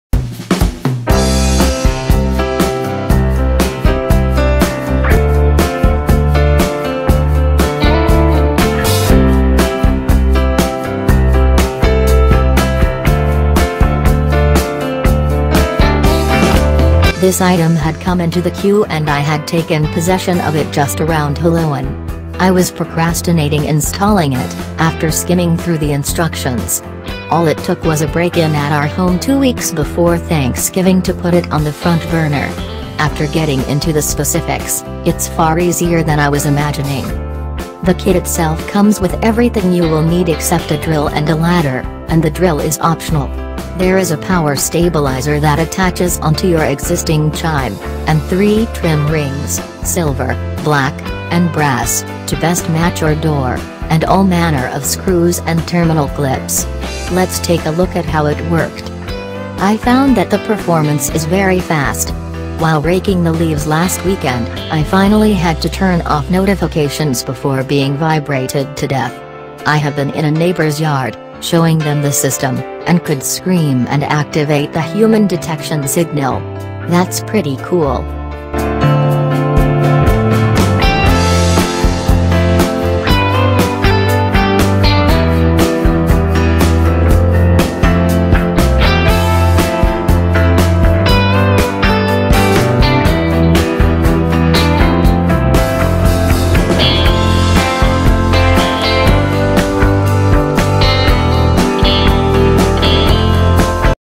Wisenet SmartCam D1 video doorbell review - Protect Your House - Gadget Life
Wisenet SmartCam D1 video doorbell review : Protect Your House

This item had come into the queue and I had taken possession of it just around Hallowen. I was procrastinating installing it, after skimming through the instructions. All it took was a break-in at our home two weeks before Thanksgiving to put it on the front burner. After getting into the specifics, it’s far easier than I was imagining.

The kit itself comes with everything you will need except a drill and a ladder, and the drill is optional. There is a power stabilizer that attaches onto your existing chime, and three trim rings (silver, black, and brass) to best match your door, and all manner of screws and terminal clips. Let’s take a look at how it worked.

I found that the performance is very fast. While raking the leaves last weekend, I finally had to turn off notifications before being vibrated to death. I have been in a neighbor’s yard, showing them the system, and could scream and activate the “human detection” signal. That’s pretty cool.

Once it was installed and I got a few notices at work, I found it fits into my lifestyle very easily. I get a message on my Apple Watch, check the video, and make sure it’s something I am expecting or need to handle, all without disengaging from a customer on the phone. If my Lovely Bride calls to ask, I am ready to let her know, after checking our expected deliveries and the doorbell video. I’m actually looking forward to salespeople coming by this spring, just so I can turn them away!

Positives
Much easier to do install and use than to read directions about
Everything is provided and explained in detail
Software is friendly, has many options, and works as expected

Negatives
There is a registration and on-going fee for video storage. Granted, this is fast becoming an expected thing, but it’s just one more “only $5 a month” service.